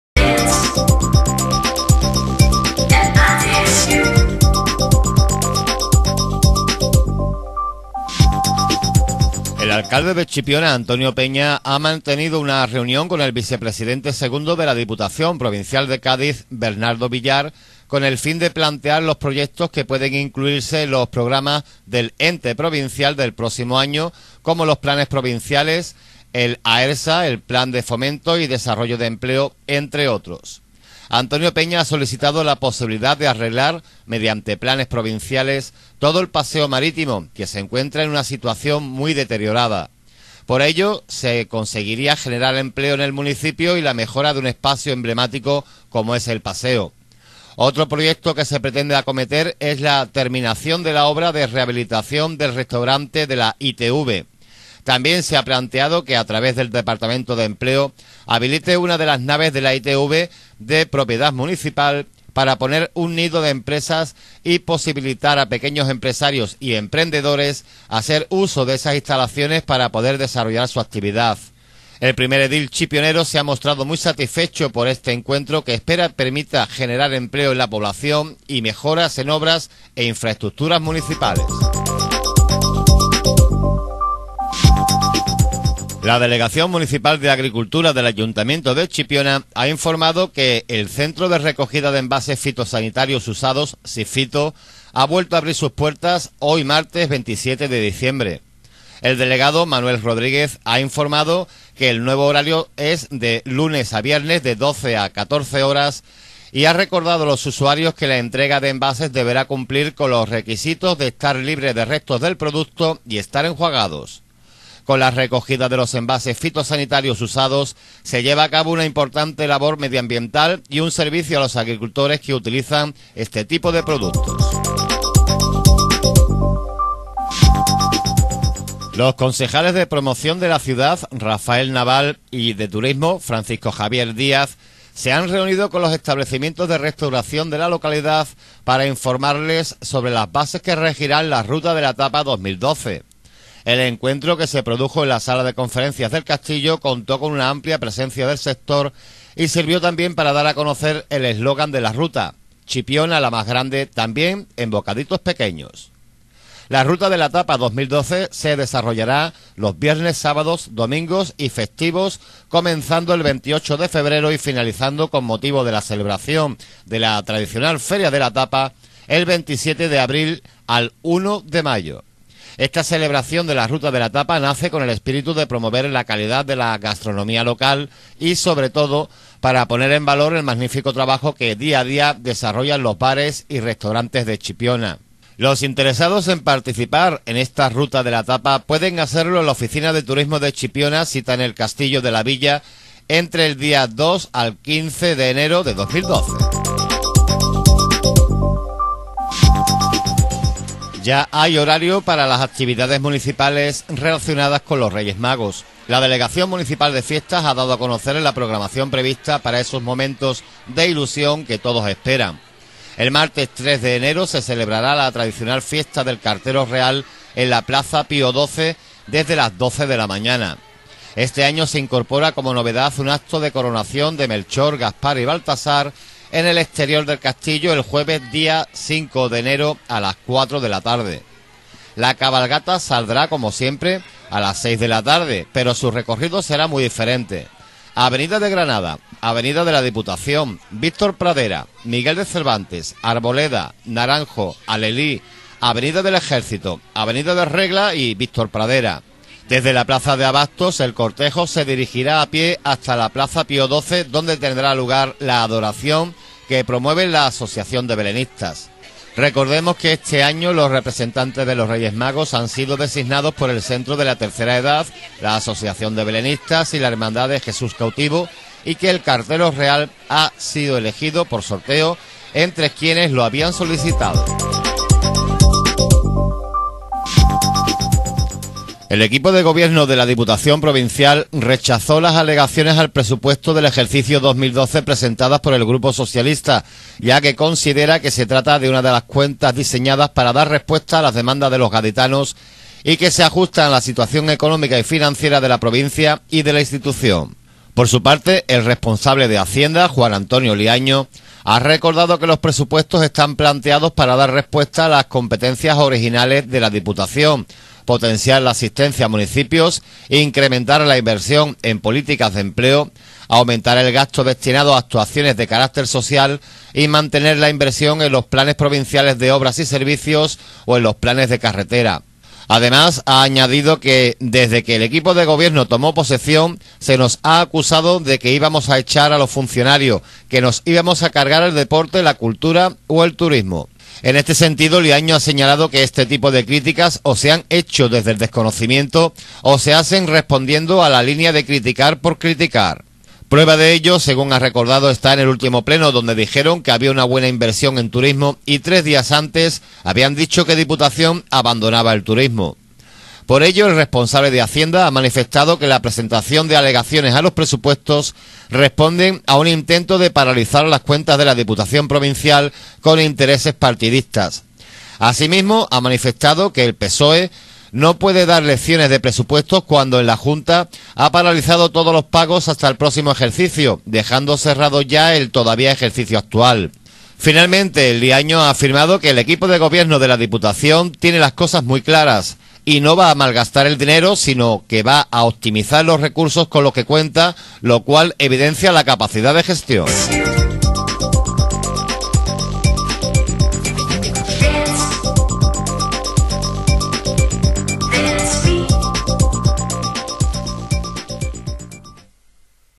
INFORMATIVO CHIPIONA SOL TV MARTES 27 DIC 11
CHIPIONA

1.- PEÑA PLANTEA PROGRAMAS DE EMPLEO

2.- VUELVE A FUNCIONAR EL CENTRO DE RECOGIDA DE ENVASES FITOSANITARIOS

3.- "CHIPIONA, LA MAS GRANDE EN BOCADITOS PEQUEÑOS" ES EL SLOGAN DE LA RUTA DE LA TAPA

4.- LA CORONACION DE LOS REYES MAGOS, UNA NOVEDAD DE ESTA NAVIDAD

Diputación:

1.- EL EQUIPO DE GOBIERNO DE DIPUTACION RECHAZÓ LAS ALEGACIONES A LOS PRESUPUESTOS